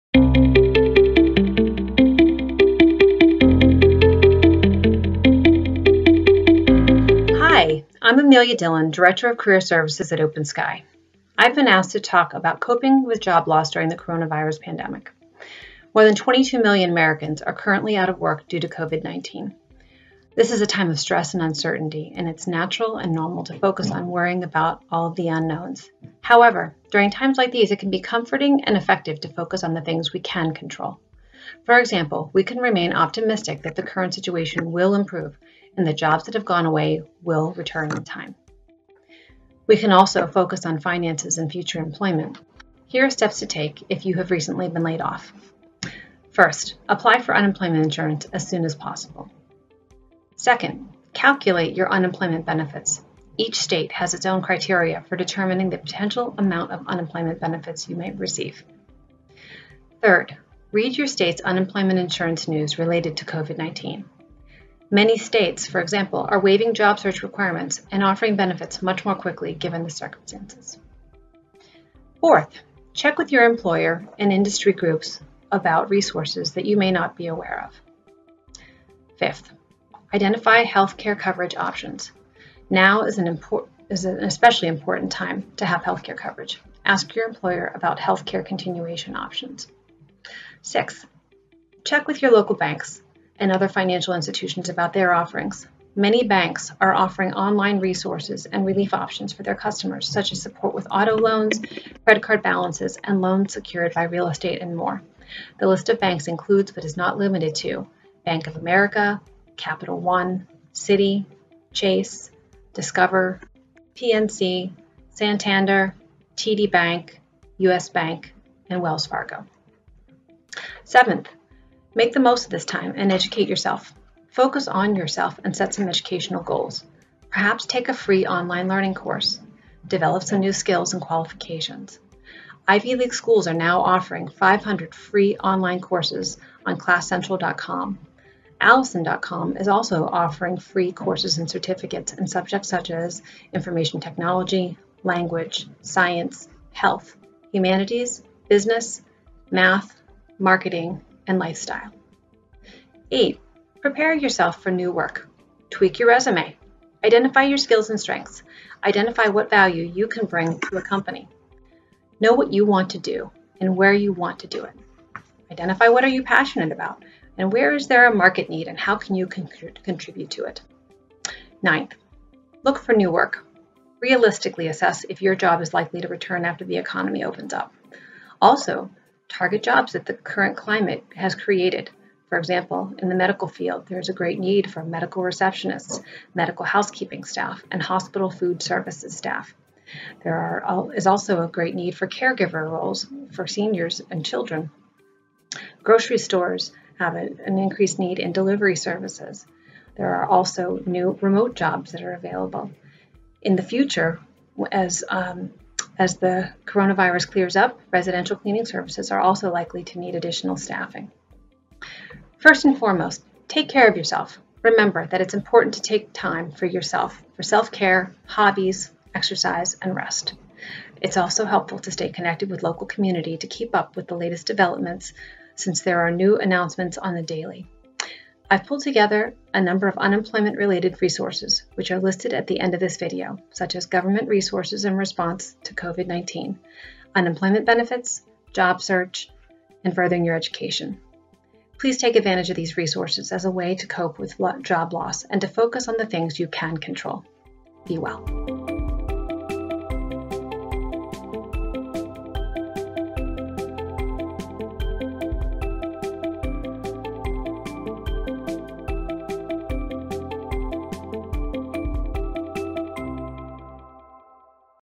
Coping During COVID 13 FINAL-Coping with Job Loss- Amelia Dillon
Amelia Dillon- Director of Career Services